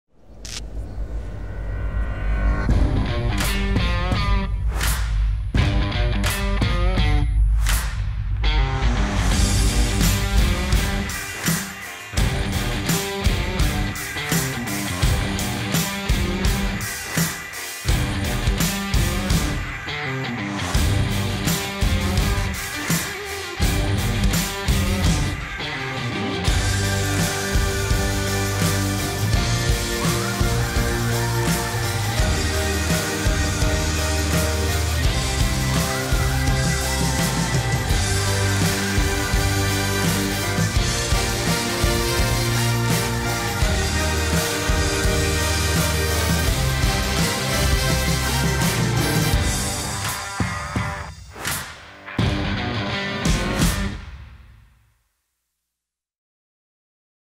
New Arsenal X Cavallaro Napoli Suit season 2018/2019
Cavallaro Napoli proudly presents the New Arsenal Football Club official Suit for season 2018/2019